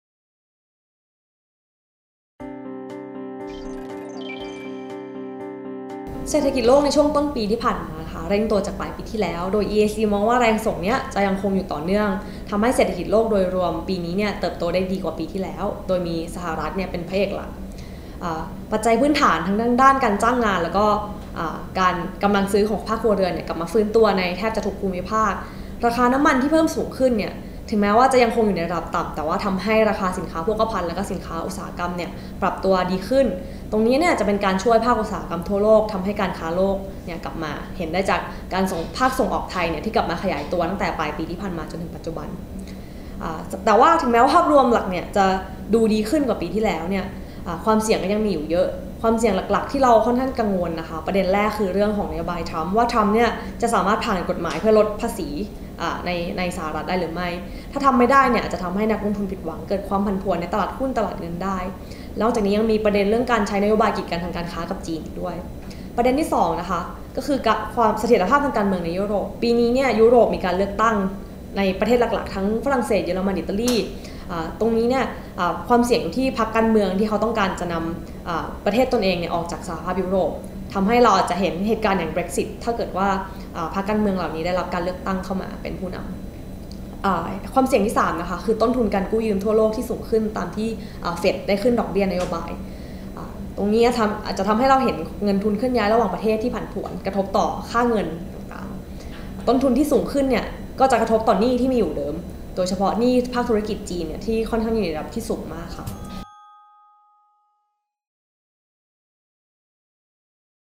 อีไอซีชี้ 3 ความเสี่ยงทางเศรษฐกิจโลกที่ต้องระวัง
อีไอซีมองว่าเศรษฐกิจโลกในช่วงต้นปีเร่งตัวจากปลายปีก่อน นำโดยสหรัฐฯ อย่างไรก็ตาม ยังคงมีความเสี่ยงสำคัญ 3 ด้านที่อาจจะฉุดรั้งเศรษฐกิจโลกให้ไม่โตตามที่คาดไว้มากนัก ดาวน์โหลดบทวิเคราะห์ฟรี